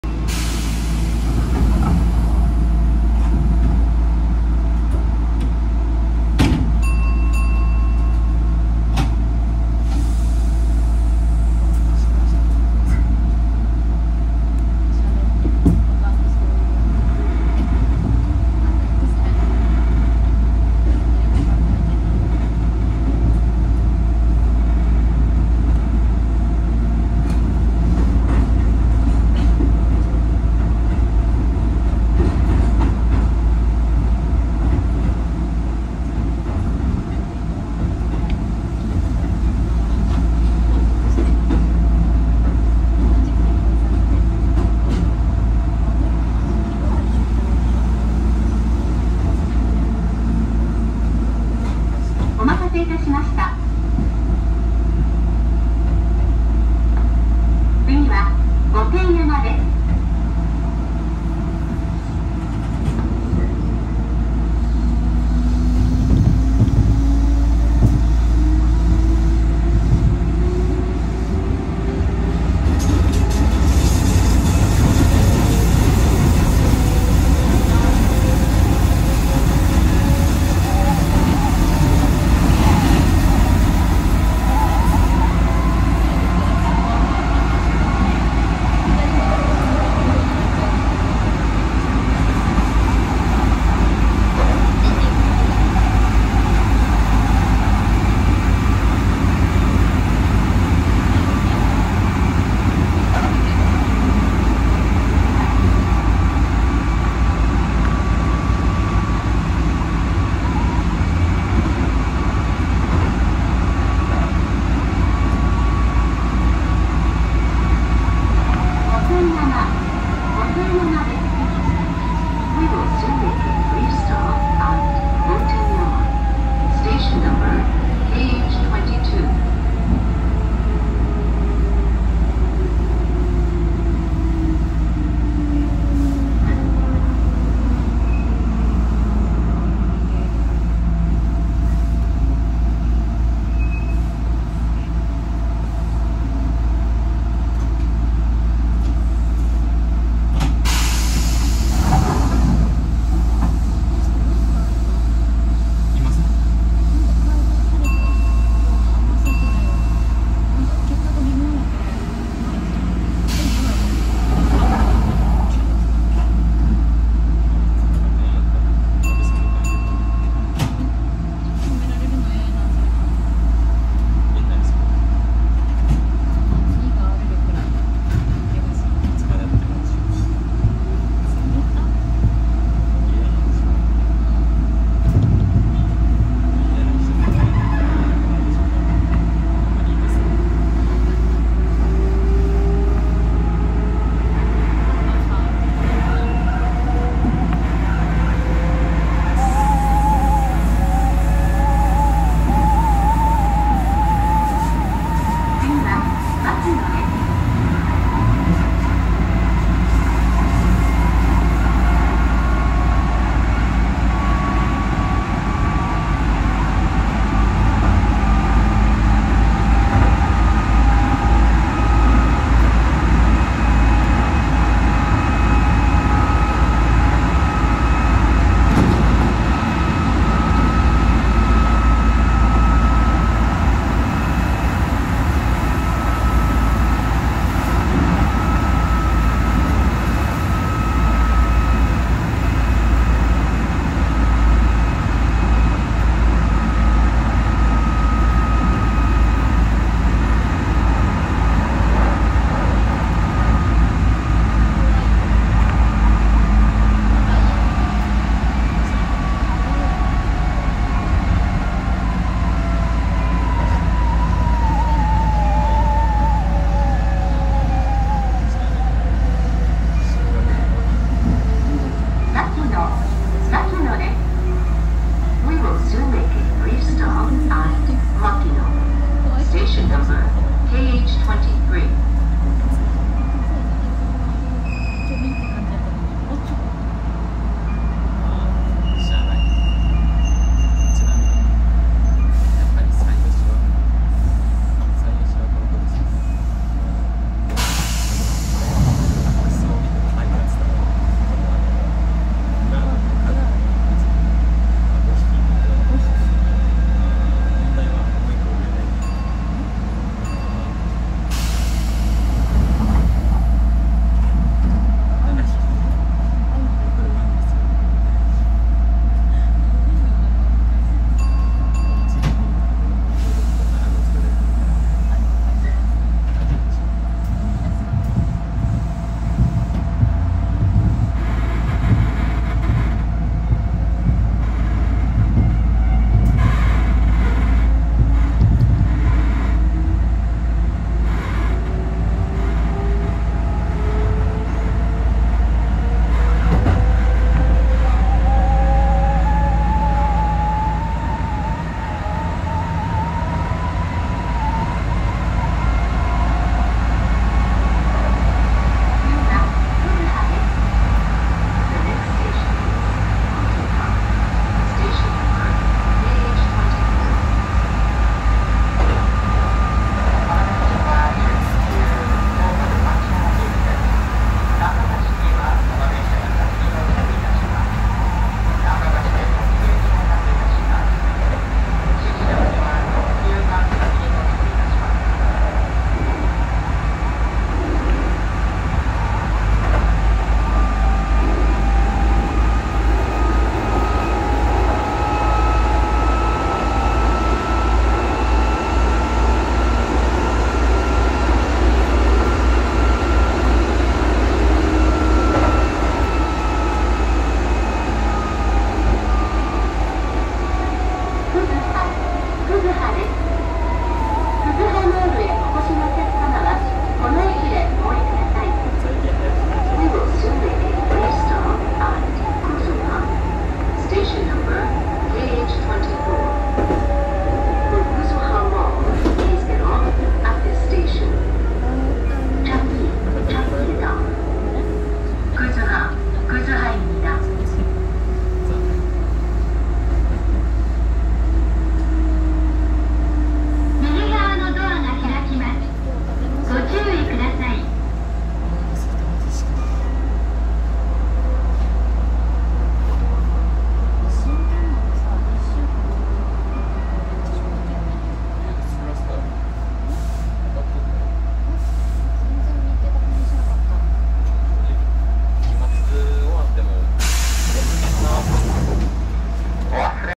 唸る爆音モーター！京阪2200系準急出町柳行き枚方市〜樟葉前面展望
ドアも爆音！2211fで撮影しました。
2:30 御殿山
4:56 牧野
7:46 樟葉